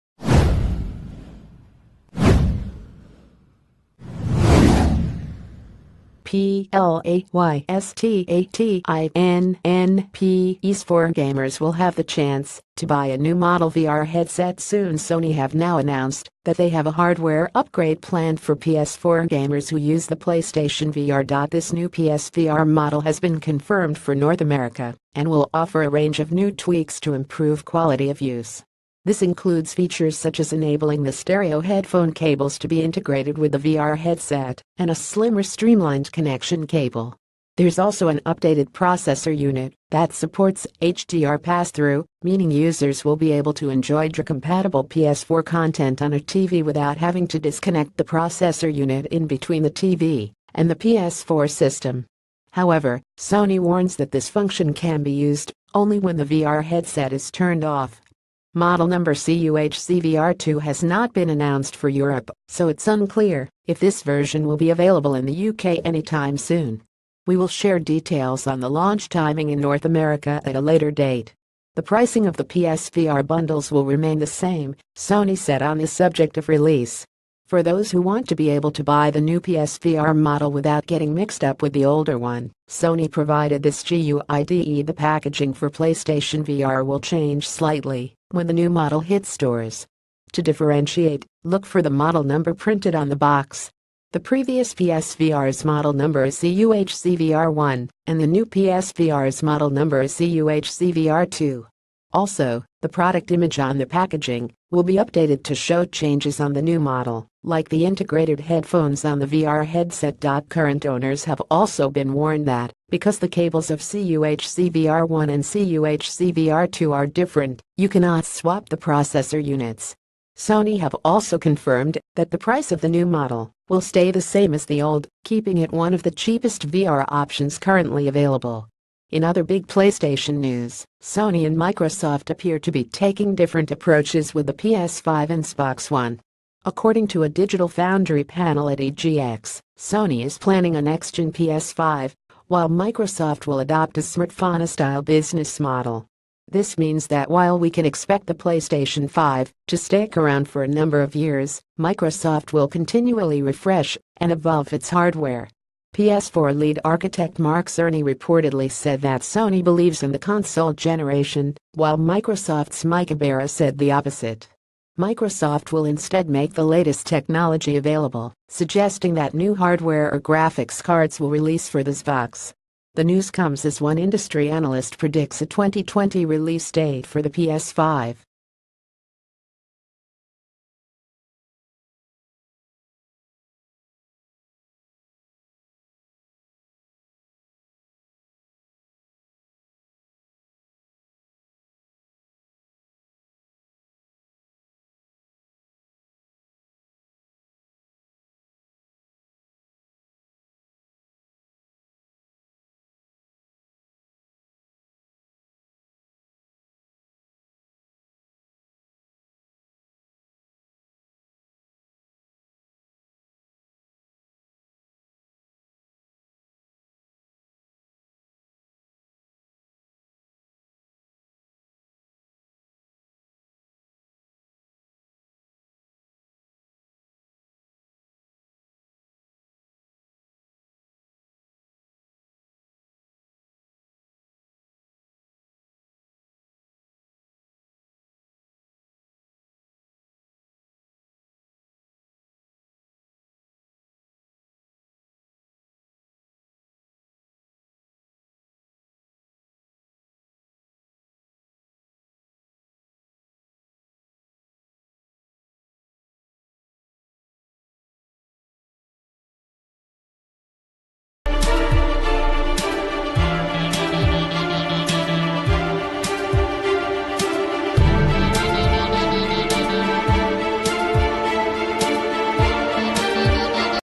Breaking News | Ps4 games boost as playstation announce new hardware
Ps4 games boost as playstation announce new hardware

PLAYSTATIONPS4 gamers will have the chance to buy a new model VR headset soonSony have now announced...